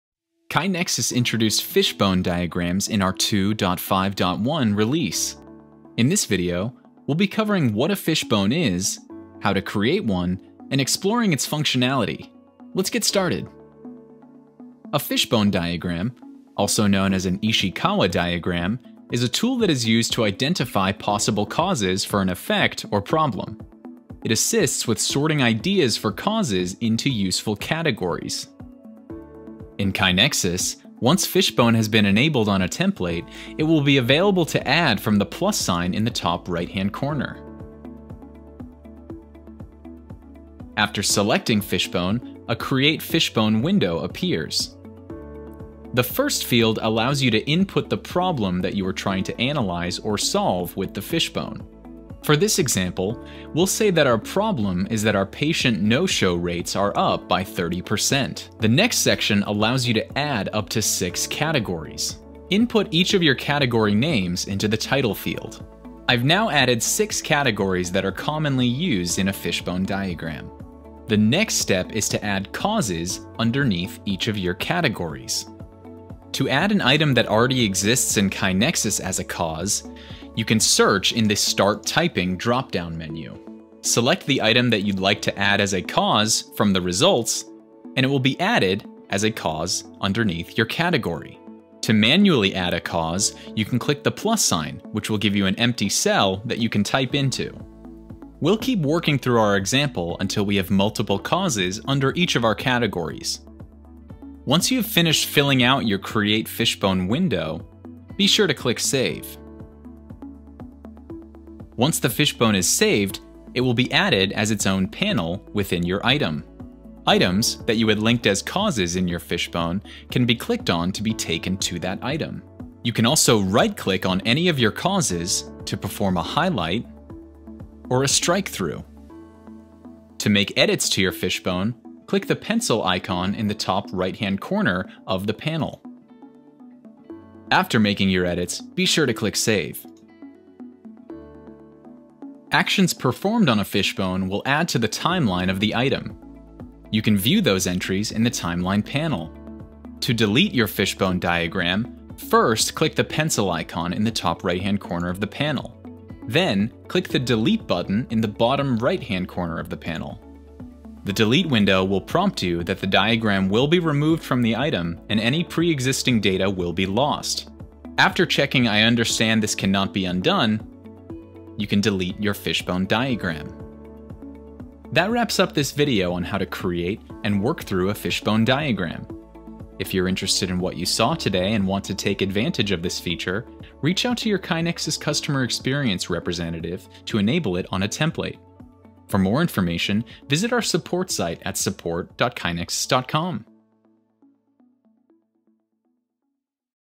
KaiNexus Fishbone Diagrams
The KaiNexus digital Fishbone Diagram is a visual management tool used to document all the potential causes of a problem to uncover the root causes and improve.